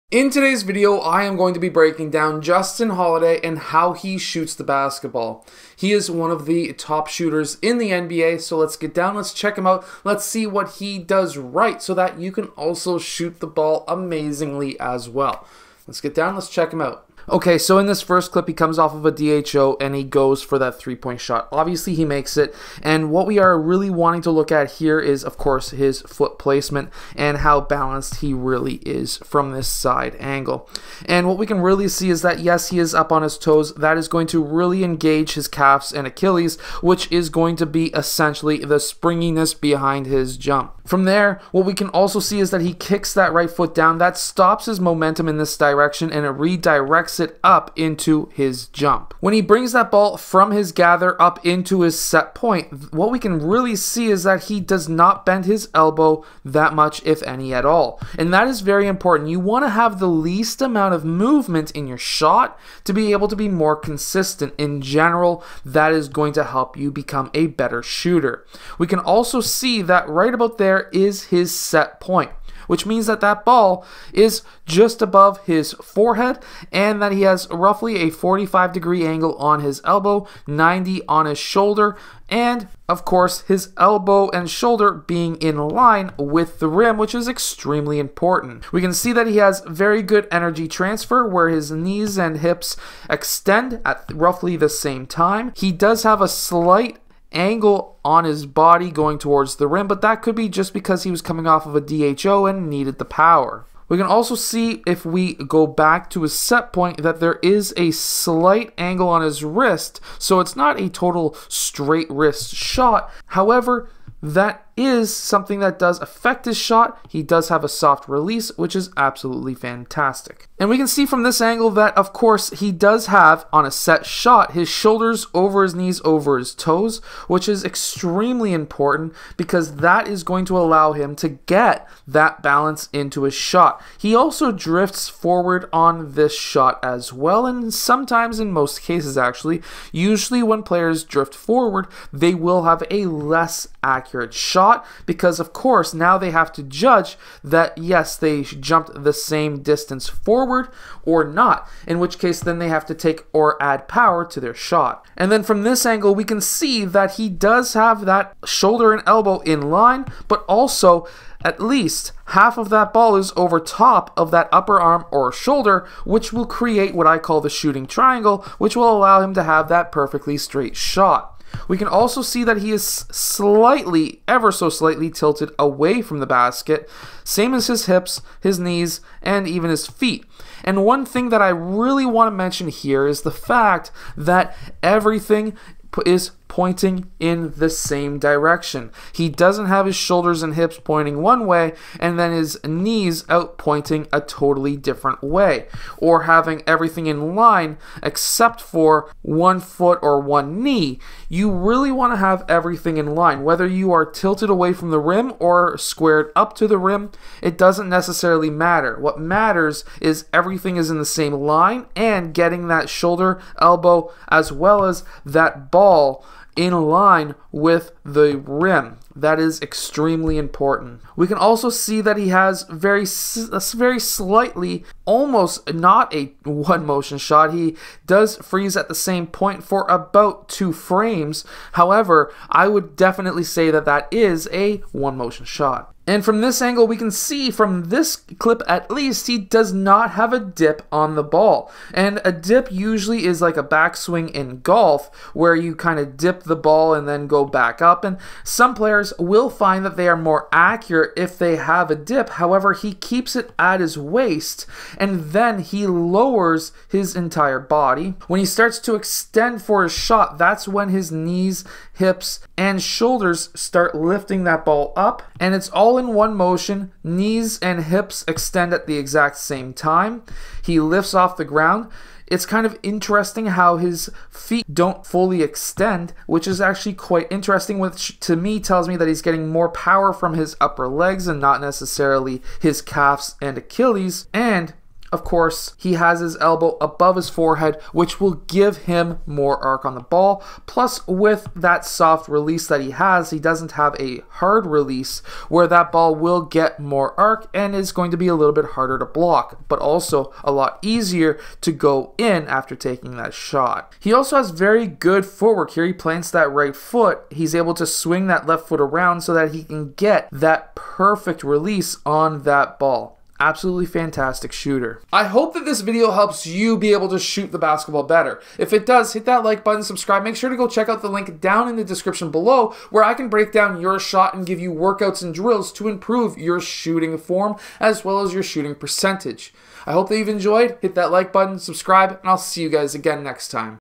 Justin Holiday Basketball Shooting Form
Justin Holiday Basketball Shooting Form, in this video I breakdown Justin Holiday and how he shoots the Basketball. Justin Holiday is one of the top shooters in the NBA and we can all learn how to shoot the Basketball better from him. I hope this video helps you become a better Basketball player and shooter.